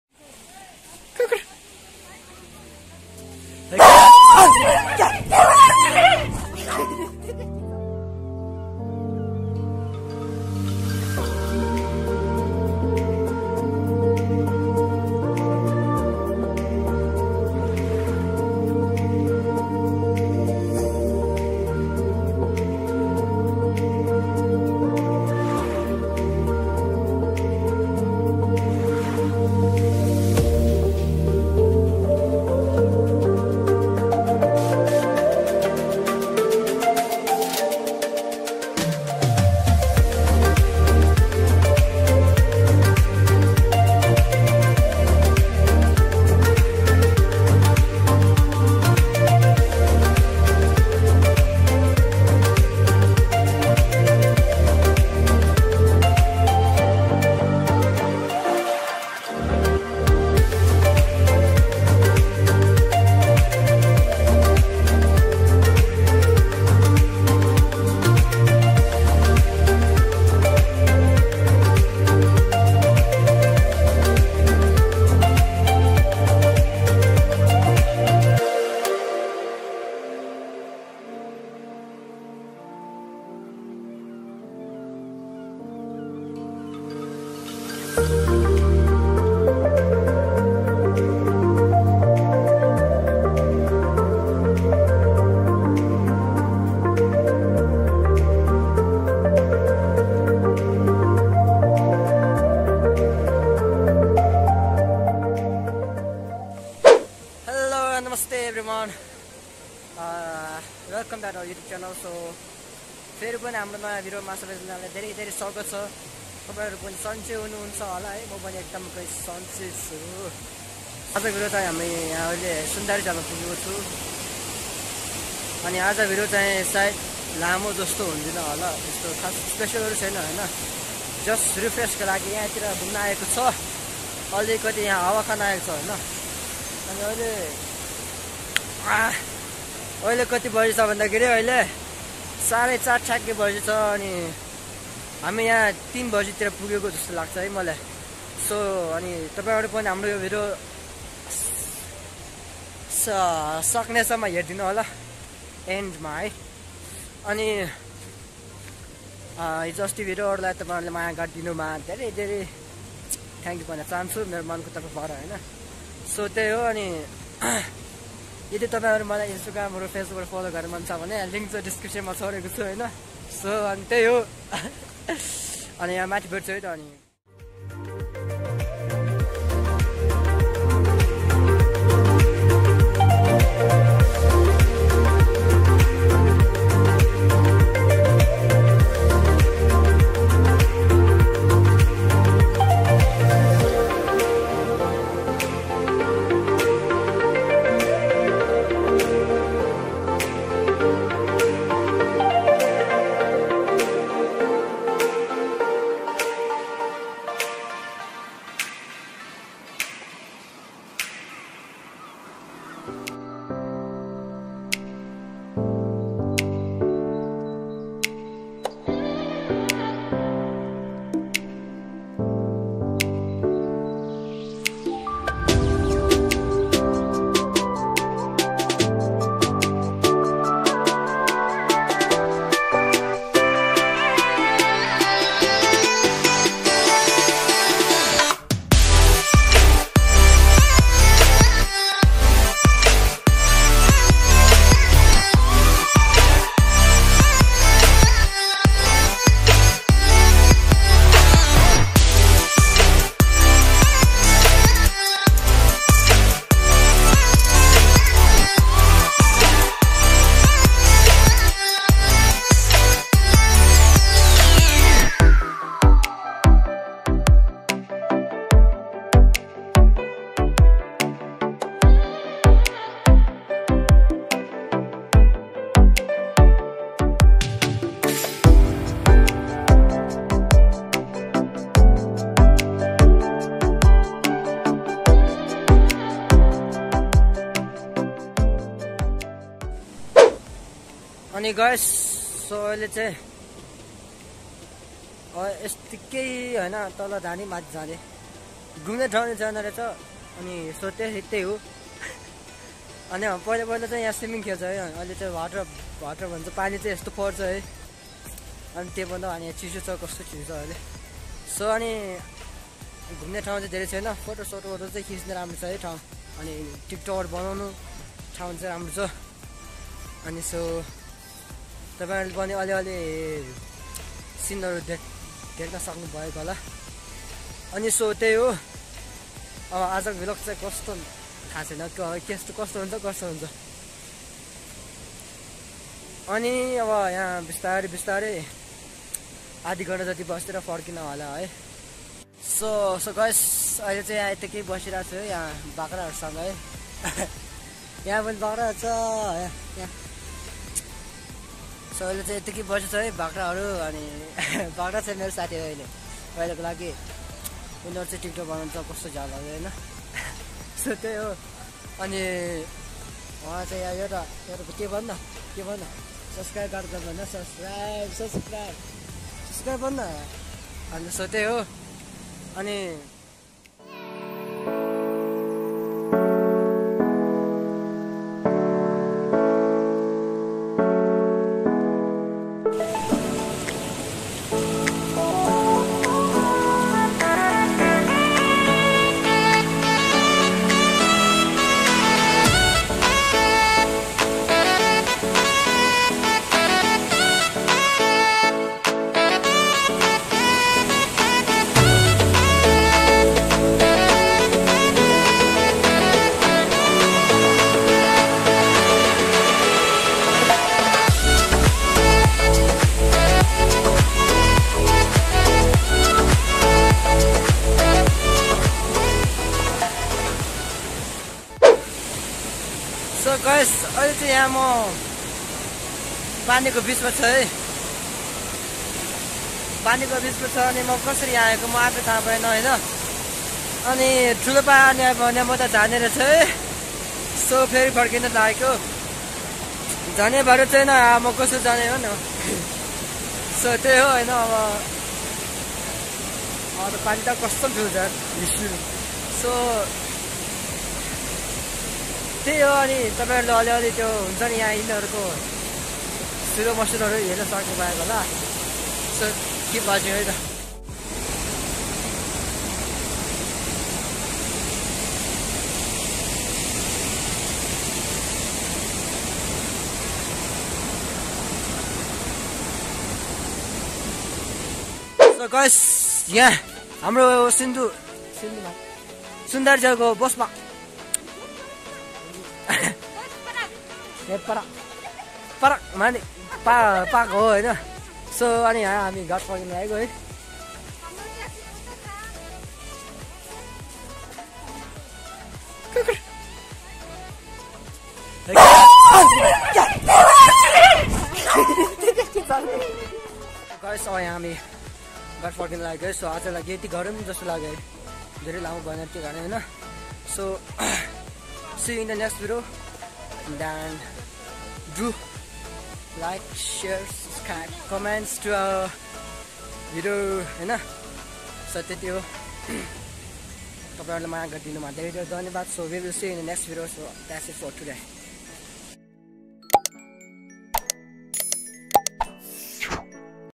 Short Vlog To SUNDARI-JAL || @DOLPO SALDANG
HELLO GUYS,THIS IS MY SHORT VLOG VIDEO TO SUNDARIJAL SO AS A MEMORY I MADE THIS VIDEO AND ALSO FOR YOU GUYS. THIS IS JUST TO REFRESH MY MIND AFTER STAYING MANY DAYS IN HOME SO THERE I AM AT SUNDARIJAL TO RESTART MY MIND. KEEP SUPPORTING AND WATCHING OUR VIDEO SO MORE INTERESTING ARE COMING SOON.

.LOVE YOU GUYS. KEEP SUPPORTING.

🟡FREE TO FOLLOW US WITH SOCIAL MEDIA🟡